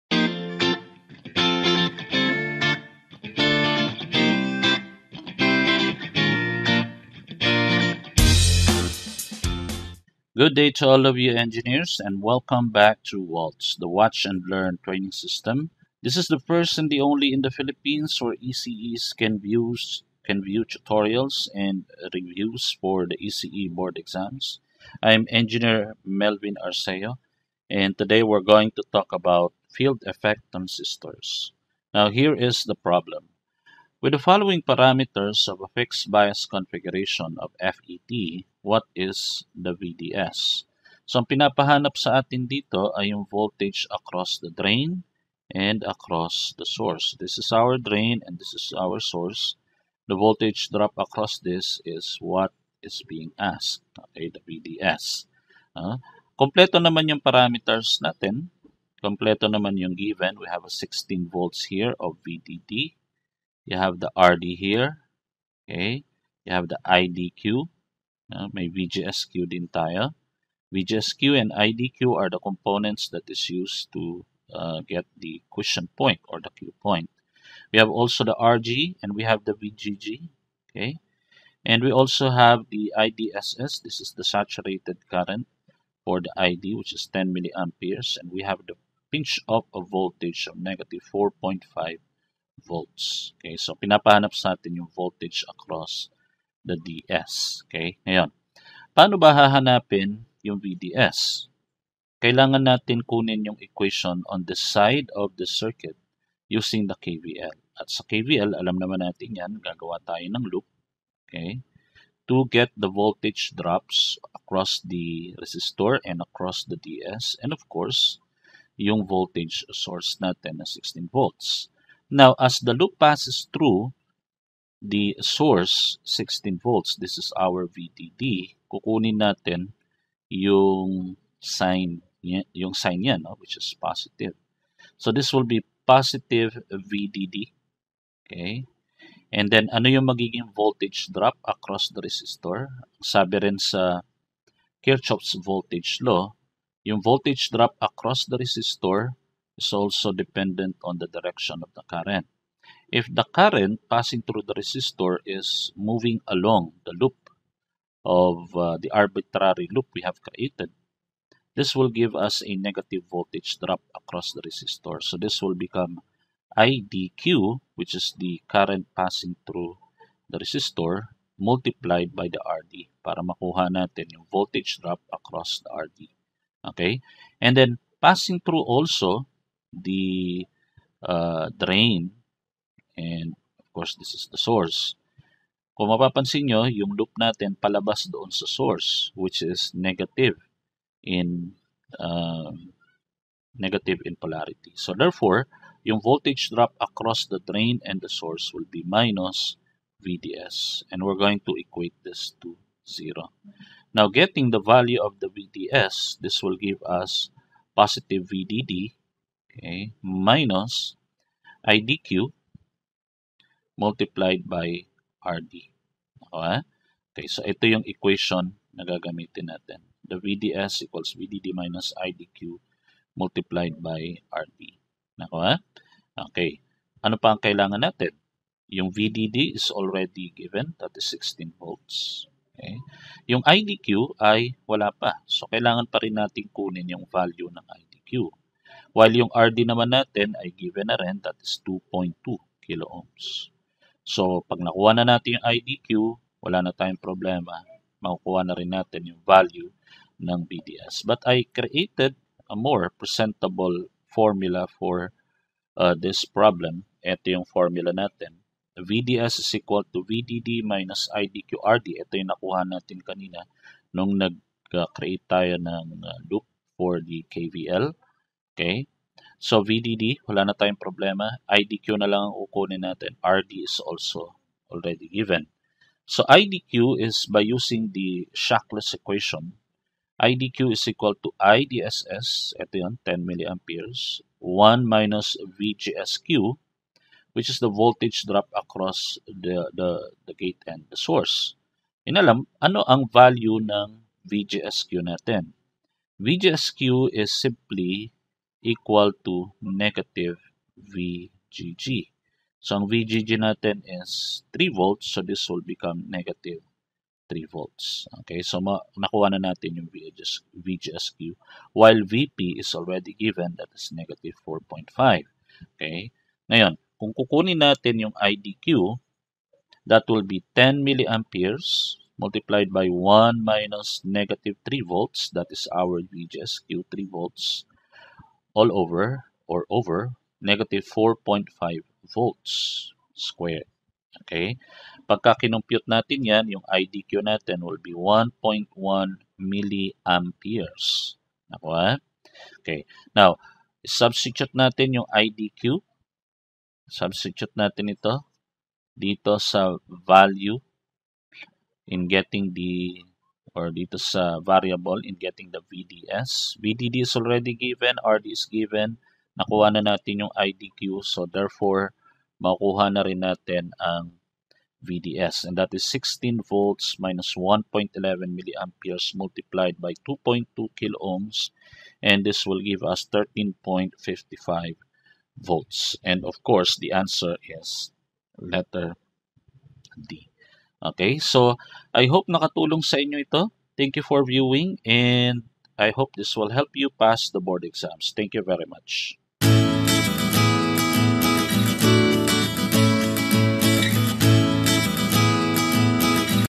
Electronics Engineering : Field Effect Transistor (FET)
Discusses problem involving Field Effect Transistors (FET) that may be helpful for ECE Board exams!